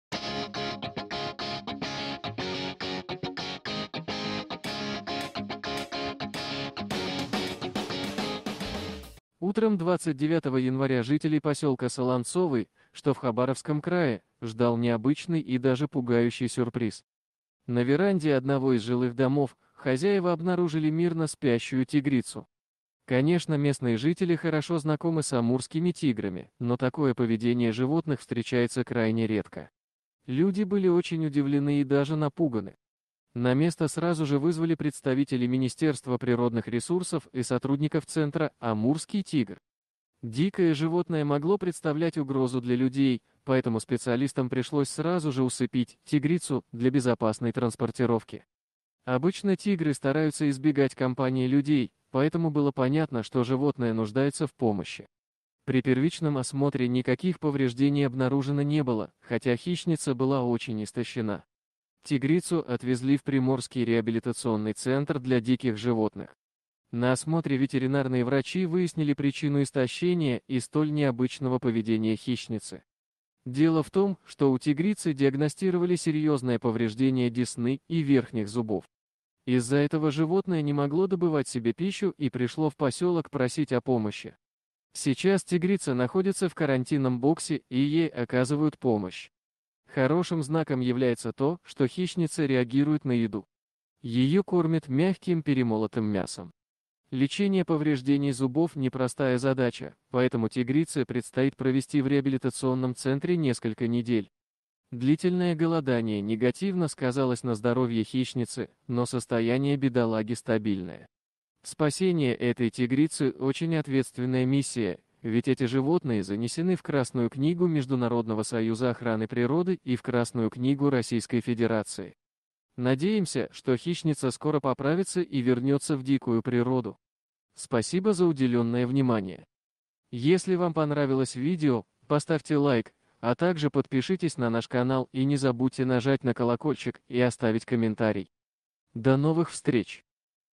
Люди обнаружили на пороге неподвижную тигрицу! Она пришла попросить о помощи!
Утром 29 января жителей посёлка Солонцовый, что в Хабаровском крае, ждал необычный и даже пугающий сюрприз. На веранде одного из жилых домов хозяева обнаружили мирно спящую тигрицу!